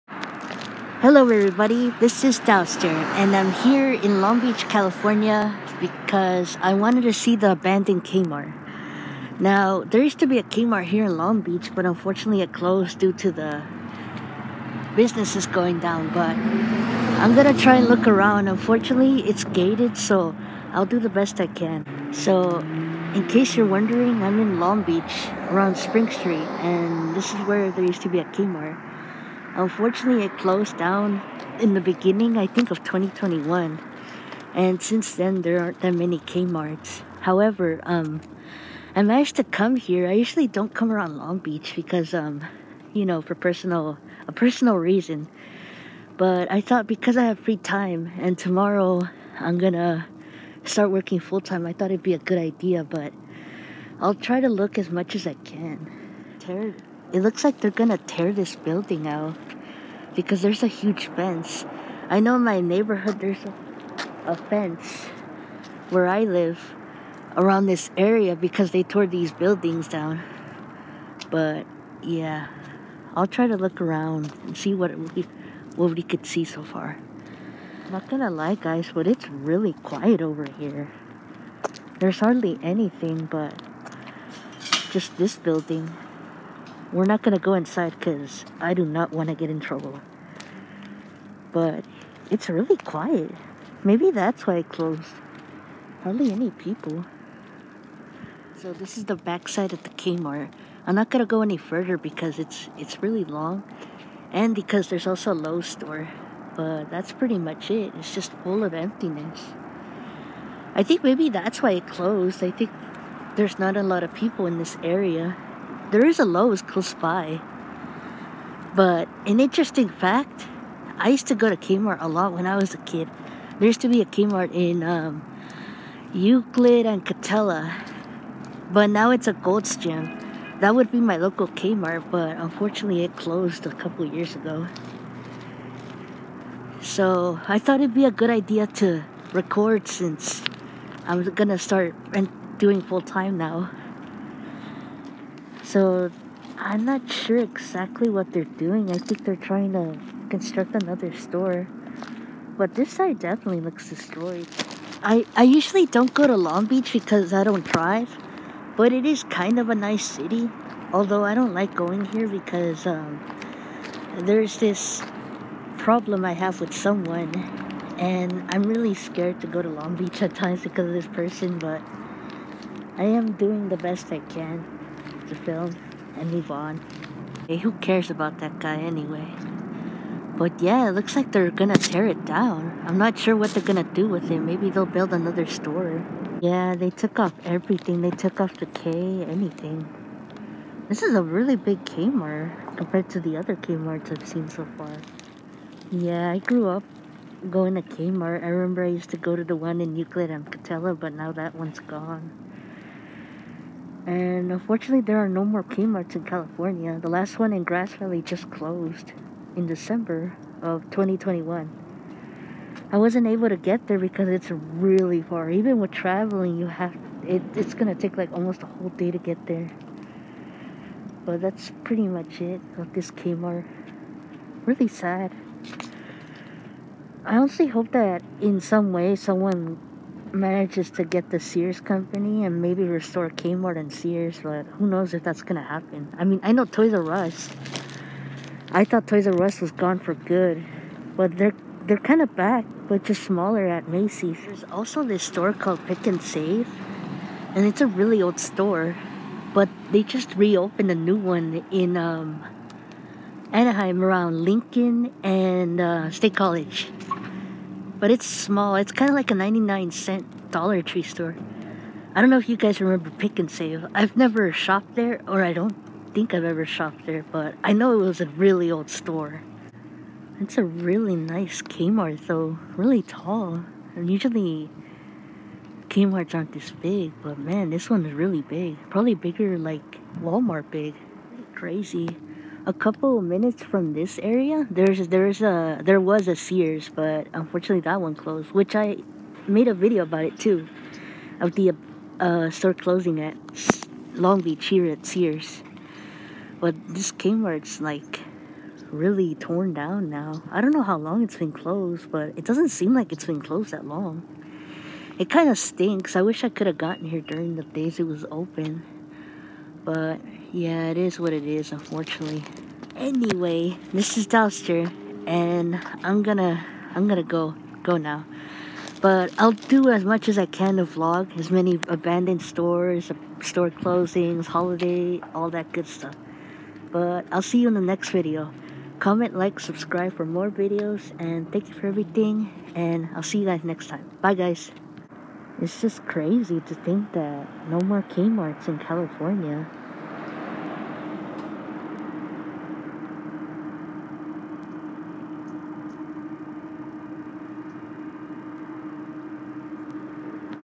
Abandoned K Mart In Long Beach, CA
This K Mart closed and is now fenced. It was scary as it was so empty.